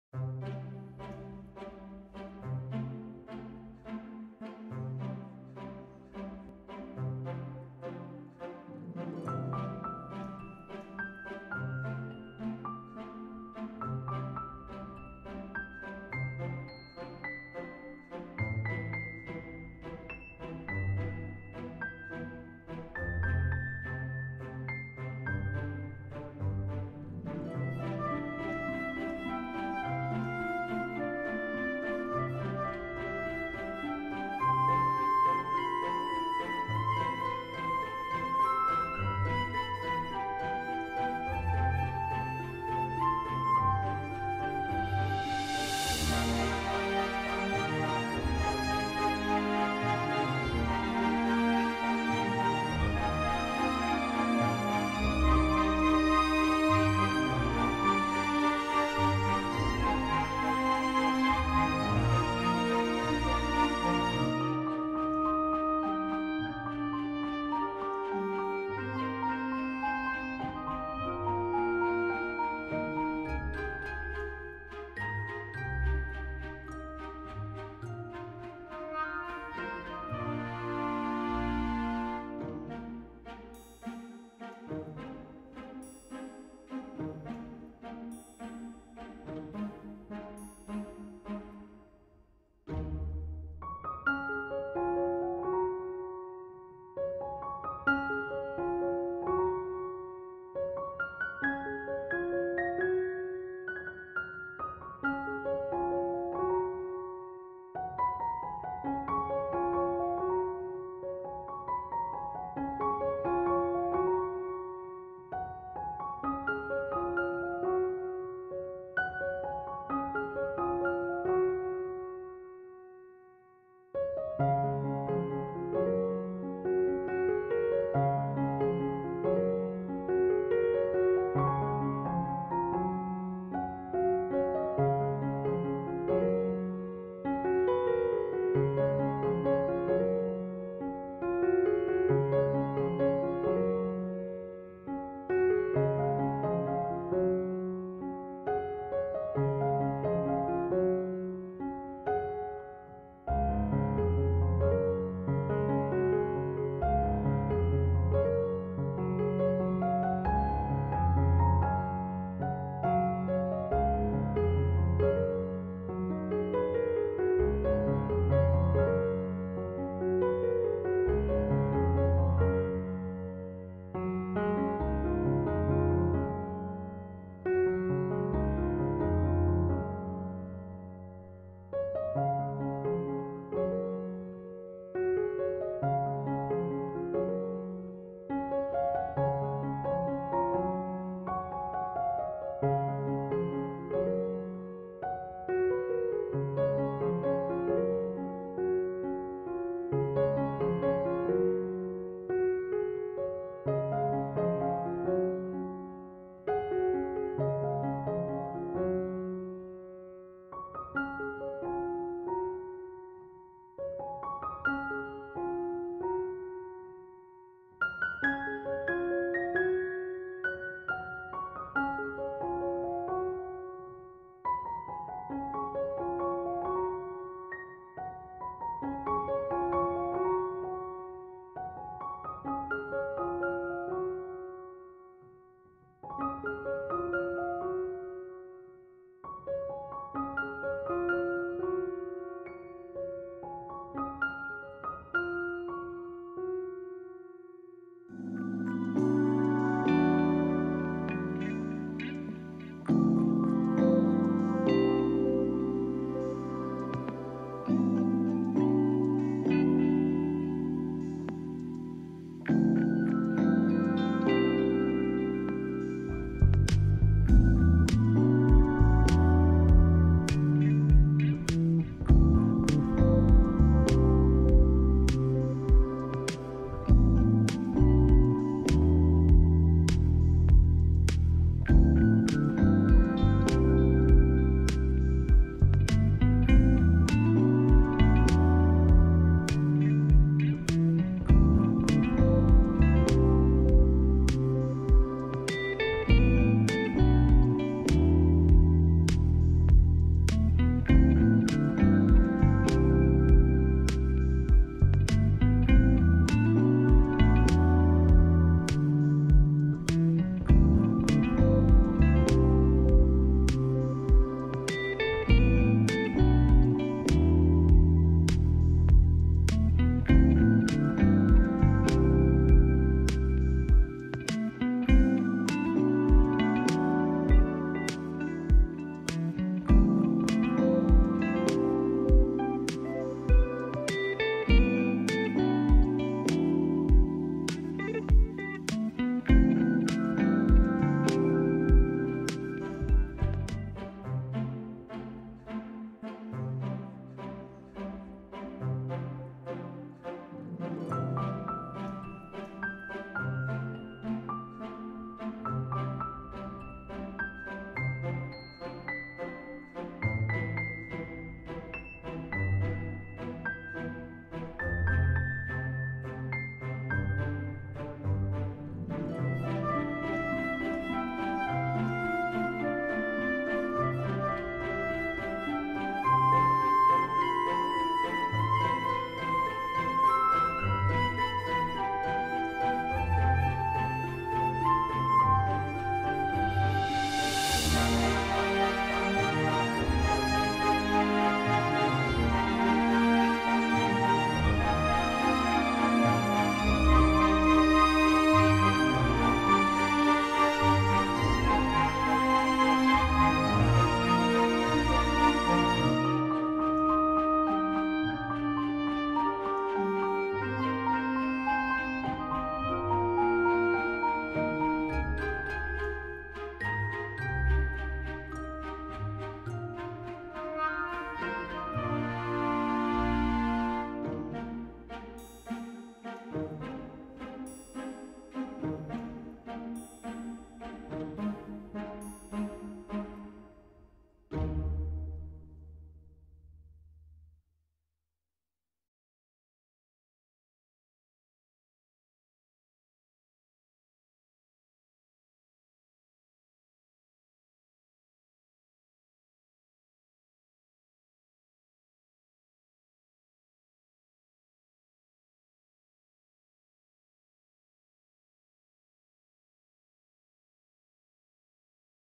Aluminum painting technique with syringes. How to paint wildflowers. Easy Acrilic art
I paint flowers with Aluminum foil and syringes. I liked the field with flowers and I decided to paint them with acrylic.